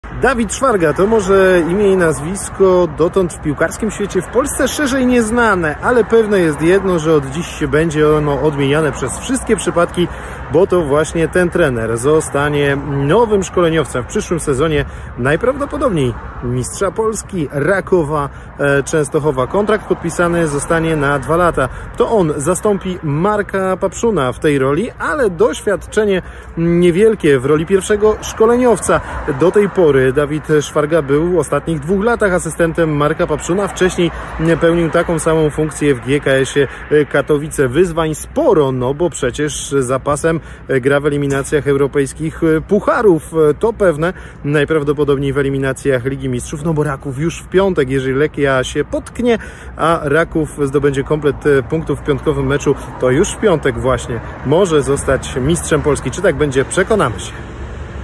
Koniec tajemnic. To on zastąpi Papszuna w Rakowie
Dawid Szwarga, dotychczasowy asystent Marka Papszuna, od nowego sezonu poprowadzi piłkarzy Rakowa Częstochowa - poinformował na konferencji prasowej właściciel klubu Michał Świerczewski. Kontrakt podpisano na dwa sezony z opcją przedłużenia o kolejne dwa.